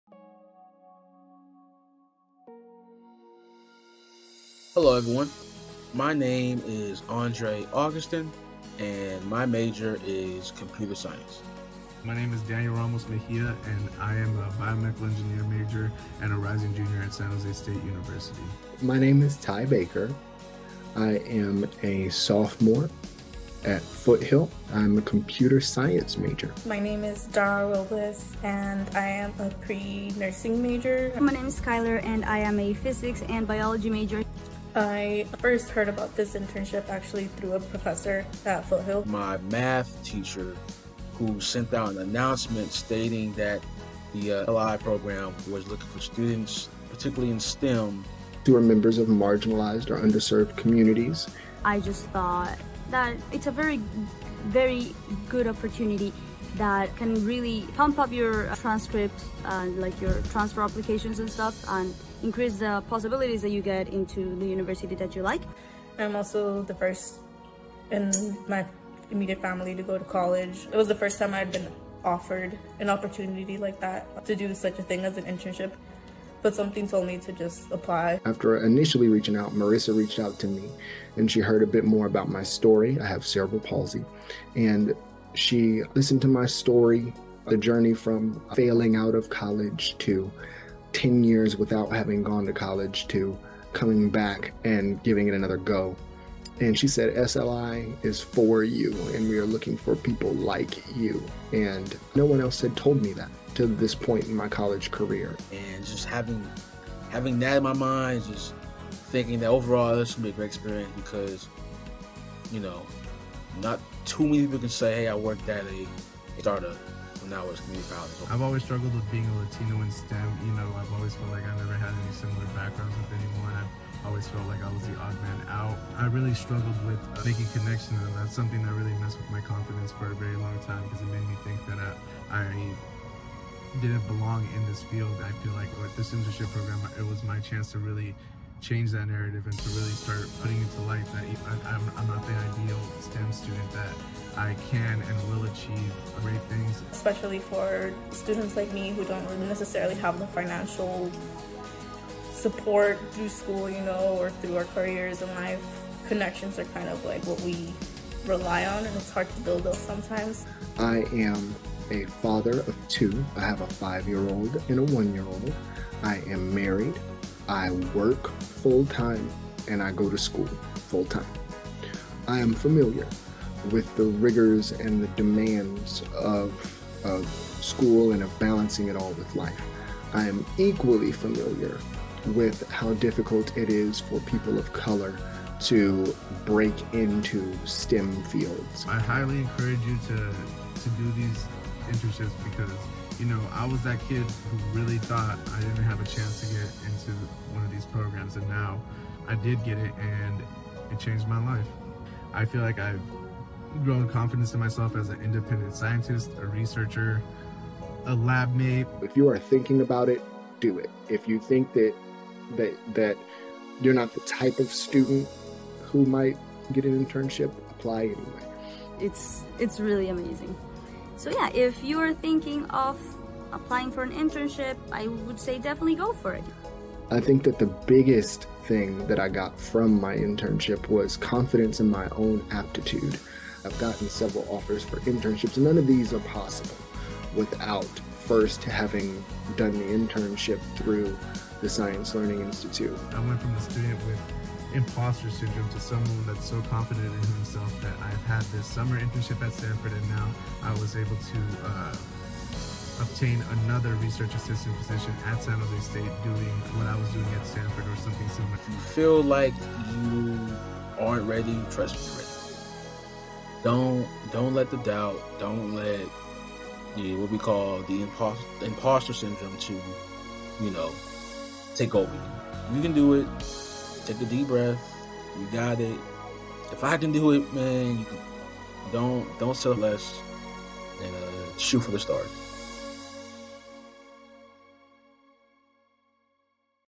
Testimonials from SLI STEM Summer Interns - summer 2022
Hear from five Foothill College students who participated in the Science Learning Institute (SLI)'s STEM Summer Internship in summer of 2022. They each speak about the impact of this experience on them as well as the imposter syndrome they overcame as students from underrepresented groups in STEM.

They make a compelling case for why students should apply for this amazing opportunity, especially those who may doubt themselves the most!

Thanks to Andre Augustin, Tyrell Baker, Schyler Martin-Rueda, Daniel Ramos, and Dara Robles for sharing their stories.